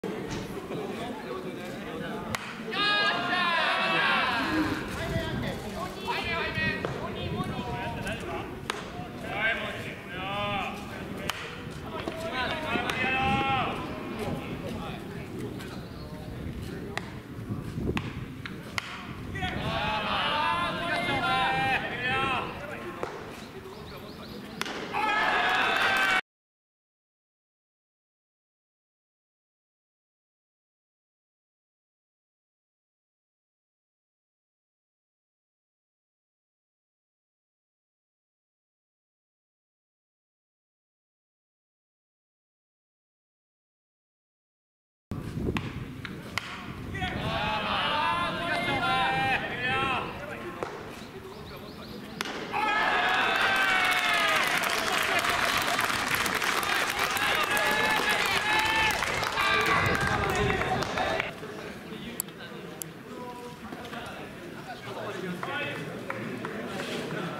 スタンドから拍手喝采！礒村嘉孝がキャッチャーフライを信じられない方法でスーパーキャッチ！
広島東洋カープ日南秋季キャンプ